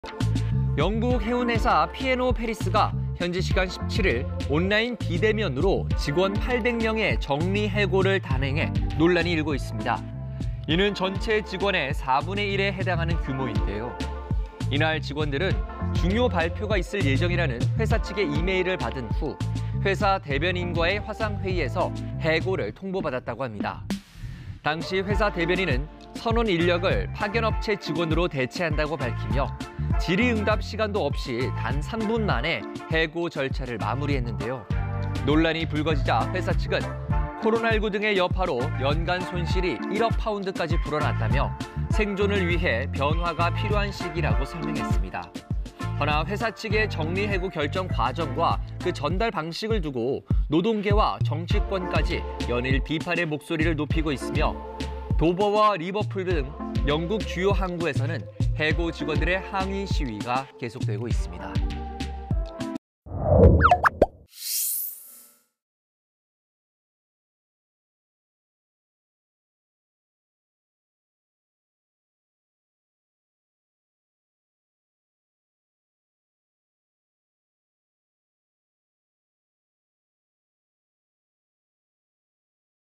[글로벌K] 화상회의로 직원 800명 해고한 영국 회사…논란 일파만파 / KBS 2022.03.22.
영국 해운회사 P&amp;O(피엔오) 페리스가 현지시간 17일 온라인 비대면으로 직원 800명의 정리해고를 단행해 논란이 일고 있습니다.

 이는 전체 직원 4분의 1에 해당하는 규모인데요.

이날 직원들은 중요 발표가 있을 예정이라는 회사 측의 이메일을 받은 후, 회사 대변인과의 화상 회의에서 해고를 통보받았다고 합니다.

 당시 회사 대변인은 선원 인력을 파견업체 직원으로 대체한다고 밝히며 질의응답 시간도 없이 단 3분 만에 해고 절차를 마무리했는데요.

논란이 불거지자 회사 측은  "코로나19 등의 여파로 연간 손실이 1억 파운드까지 불어났다며 생존을 위해 변화가 필요한 시기"라고 설명했습니다.

 회사 측의 정리해고 결정 과정과 그 전달 방식을 두고 노동계와 정치권까지 연일 비판의 목소리를 높이고 있으며 도버와 리버풀 등 영국 주요 항구에선 해고 직원들의 항의 시위도 계속되고 있습니다.

▣ 제보 하기
◇ 카카오톡 : 'KBS제보' 검색